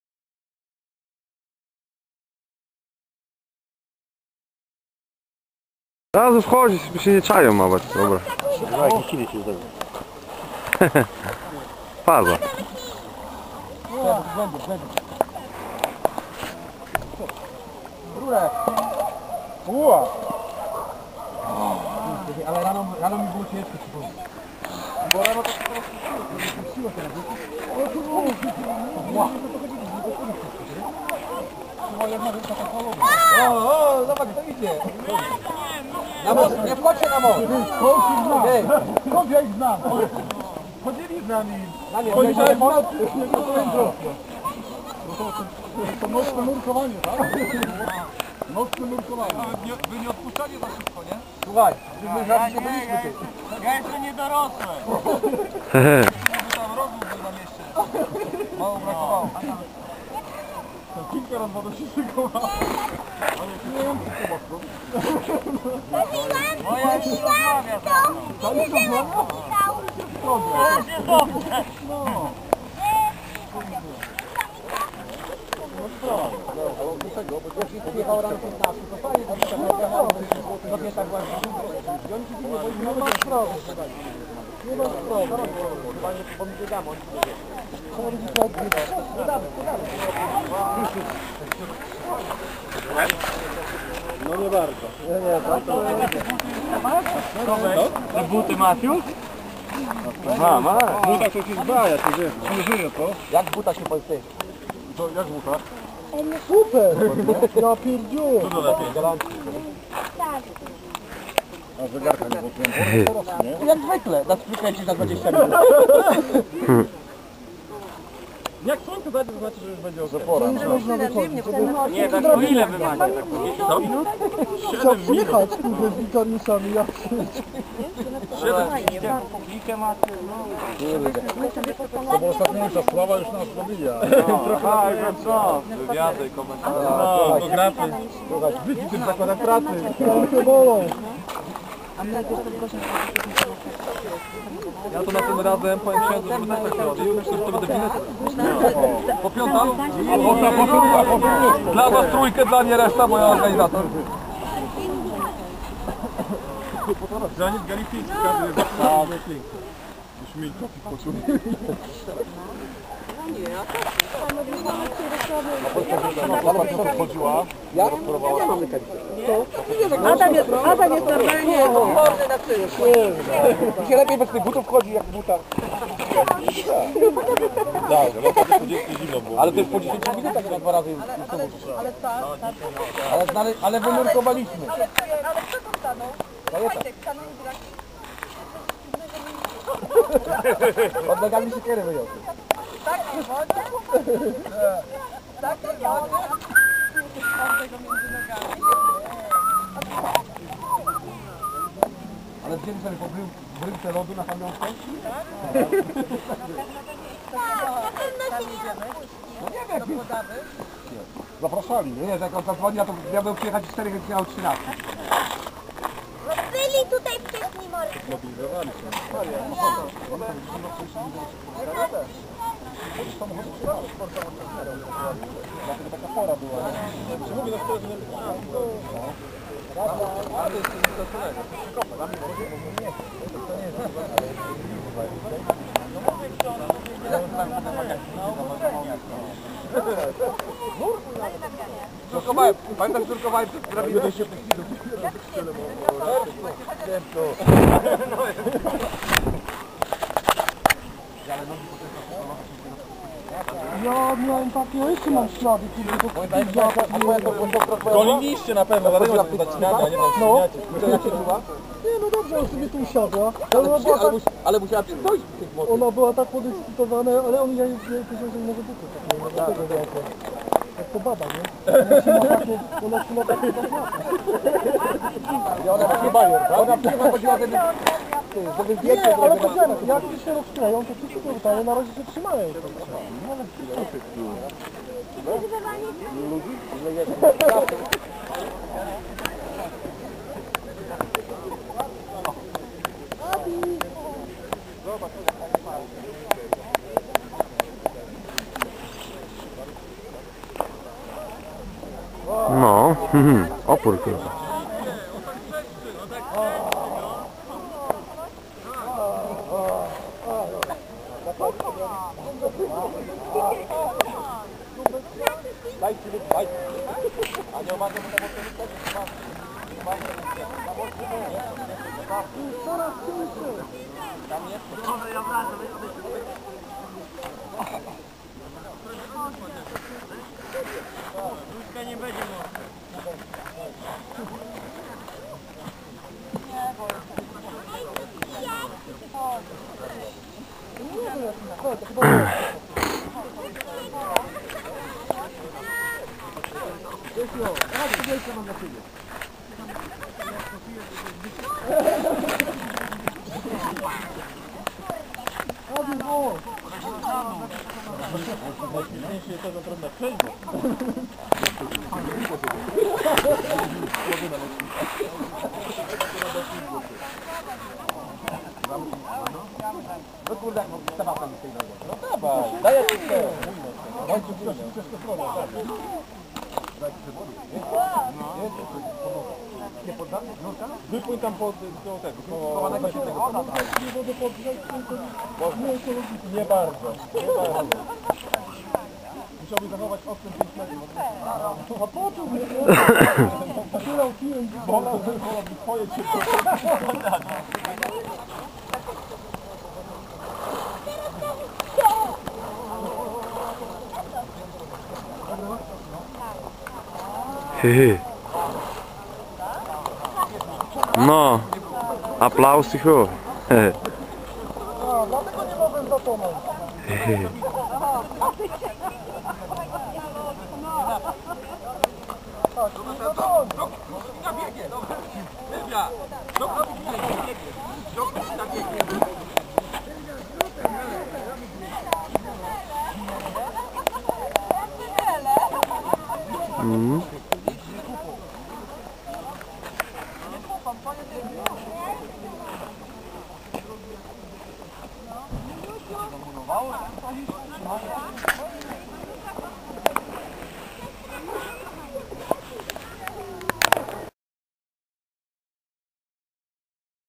Myśliborskie morsy
10 minut w lodowatej wodzie